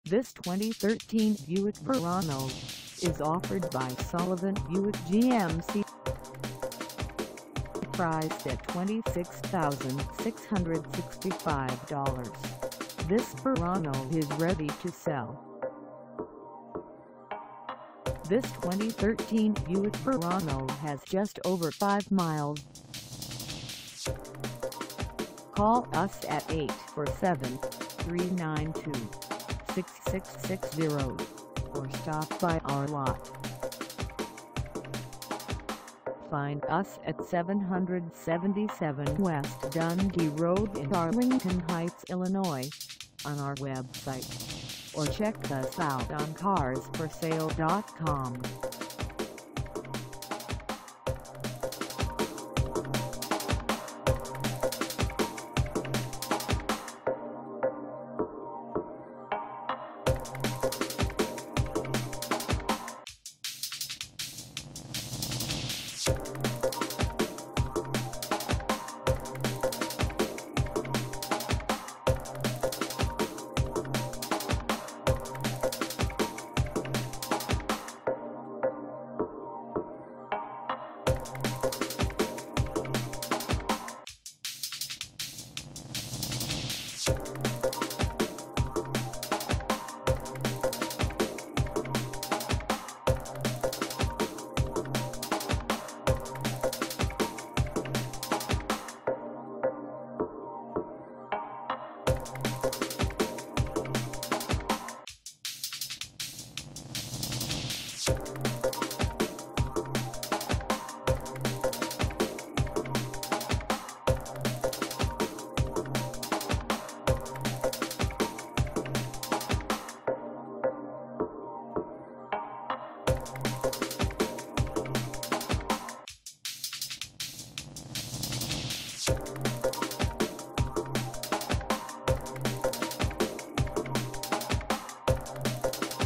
2013 Buick Verano Convenience Group - for sale in ARLINGTON
Come test drive this 2013 Buick Verano Convenience Group for sale in ARLINGTON HEIGHTS , IL.